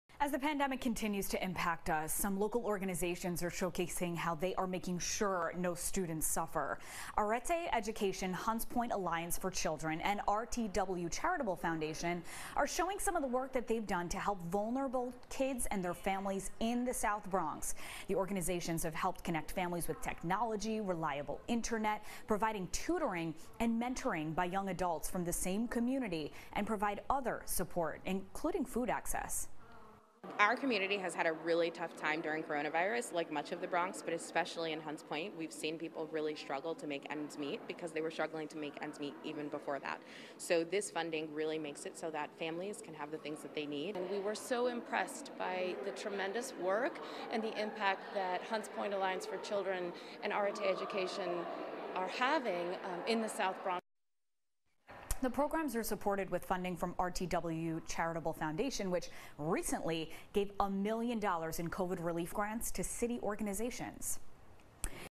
News 12 - HPAC & RTW Charitable Foundation
News 12 coverage of HPAC's work with RTW Charity Foundation during the Covid-19 Pandemic.